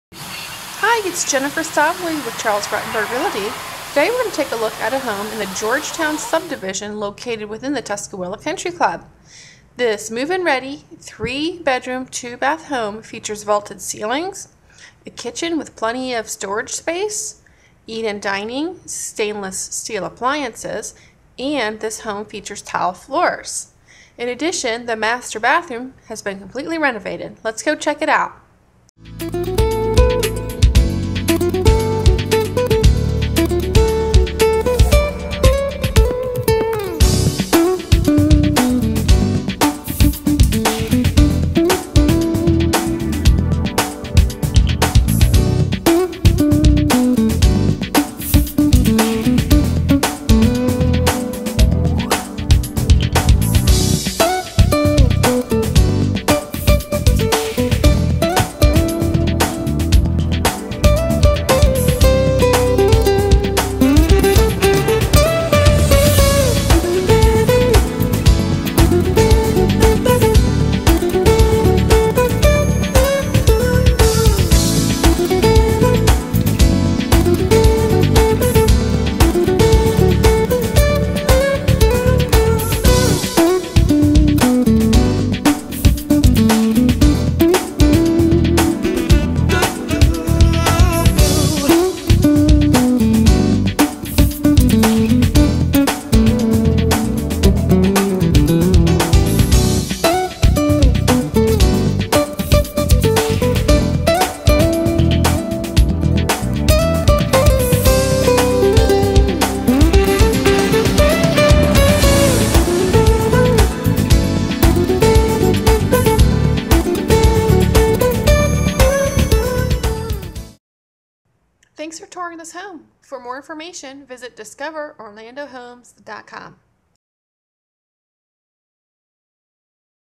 678 Barrington Circle Winter Springs, FL
Take a tour of this move in ready home located in the Georgetowne Subdivision of the Tuscawilla Country Club.  The 3 bedroom home features 18" tile floors, vaulted ceilings, renovated master bathroom with custom tile shower and new tub, a kitchen with lots of storage space and eat in dining.